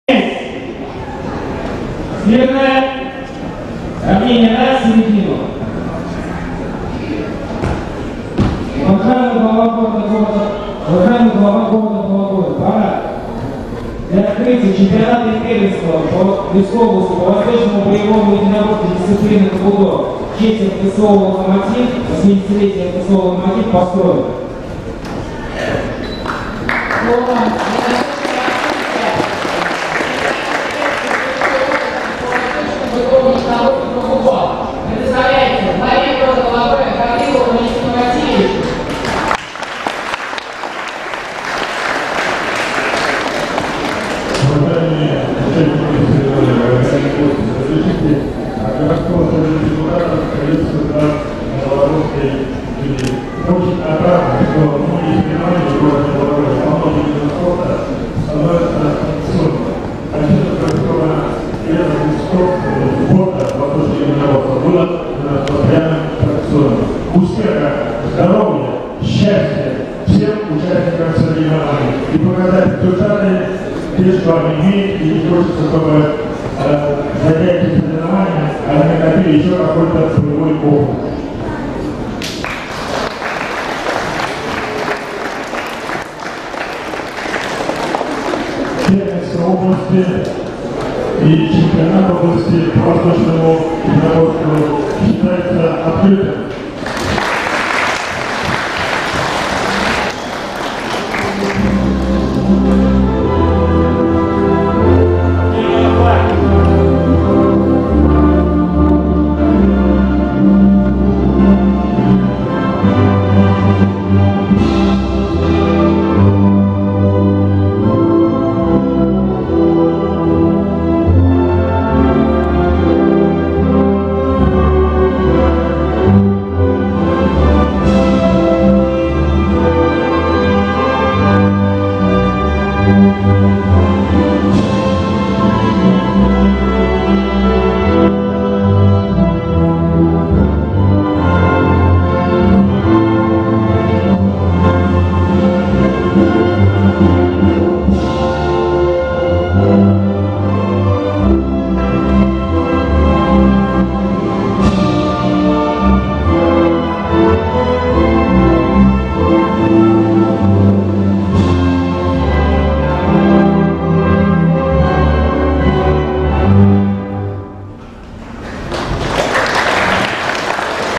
Подъём флага соревнований. Чемпионат и Первенство Тверской области по Кобудо 2016
Подъём флага соревнований Чемпионат и Первенство Тверской области по Кобудо 2016, Приветствие участников соревнований главой града Бологое - Гавриловым Валентином Васильевичем

28 февраля 2016 года

г.Бологое. Школьный переулок дом 9 литер А.
Тел.: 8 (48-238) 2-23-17, 5-44-17.